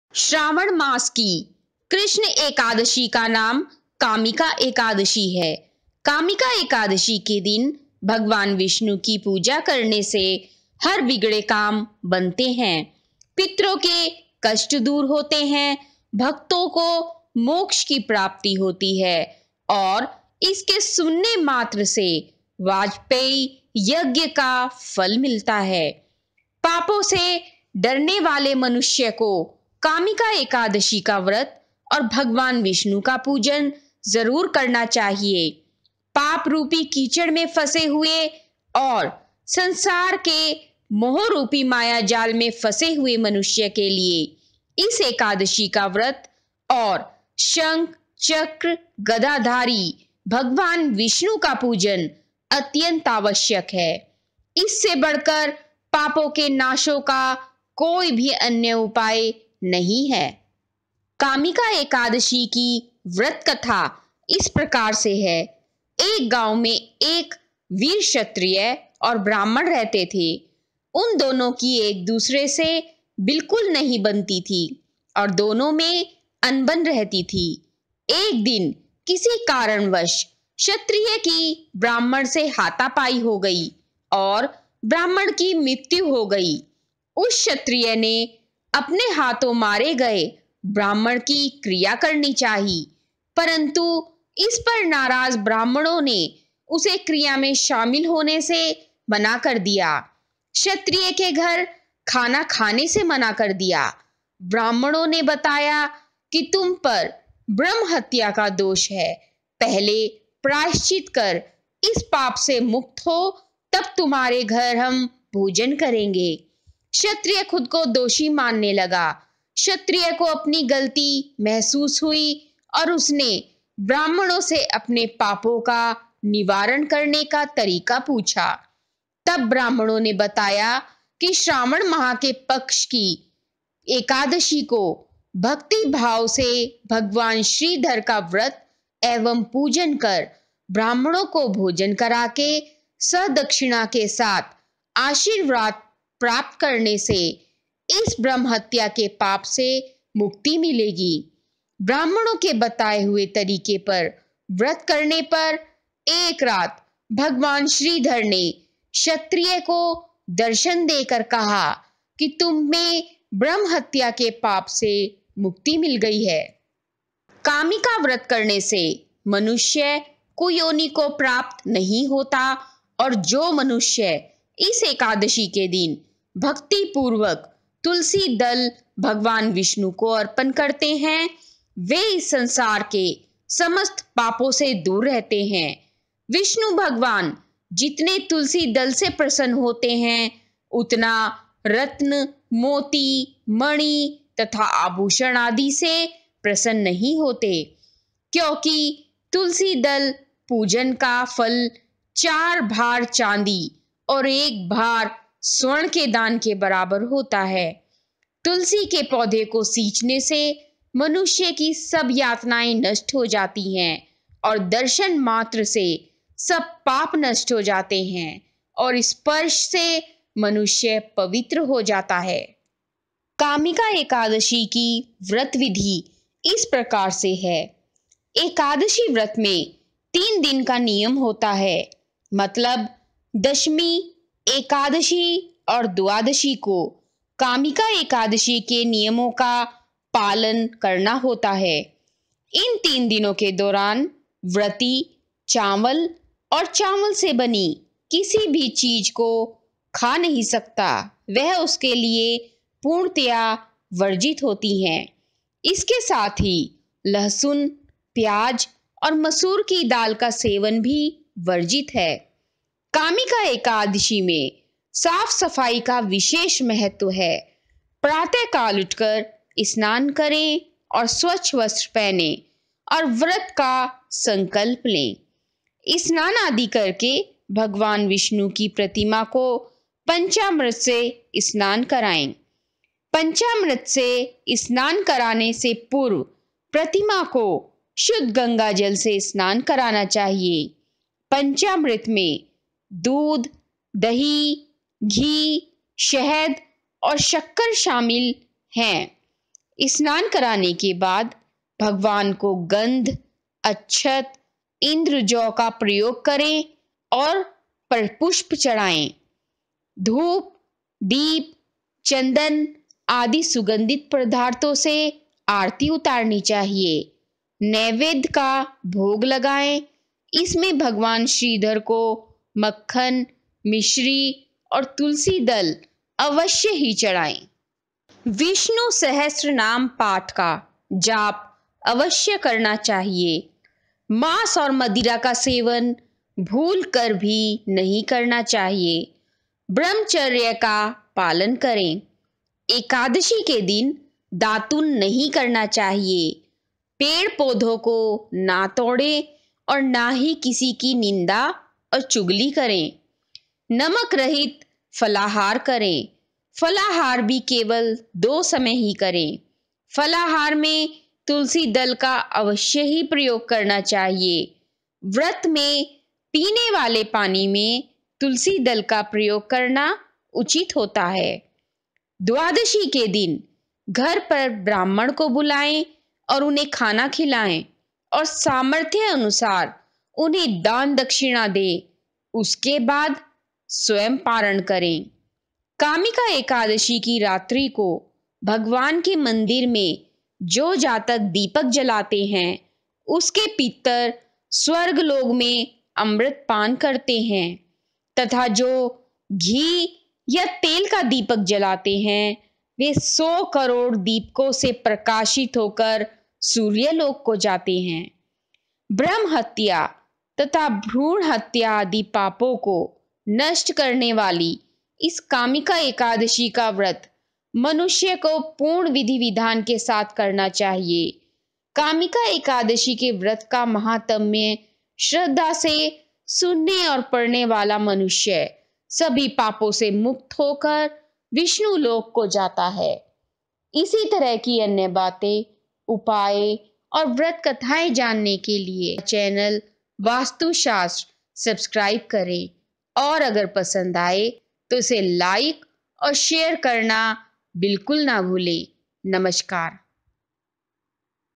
पापों से डरने वाले मनुष्यों को कामिका एकादशी का व्रत जरूर रखना चाहिए
Vastushastra omshanti एक गांव में एक वीर श्रत्रिय और ब्राह्मण रहते थे.उन दोनों की एक दूसरे से नहीं बनती थी और दोनों में अनबन रहती थी.एक दिन किसी कारण वश श्रत्रिय की ब्राहमण से हाथापाई हो गई और ब्राहमण की मृत्य हो गई.उस श्रत्रिय नेअपने हाथों मारे गये ब्राहमण की क्रिया करनी चाही,परन्तु इस पर नाराज ब्राह्मणों ने उसे क्रिया में शामिल होने से मना कर दिया,श्रत्रिय के घर खाना खाने से मना कर दिया,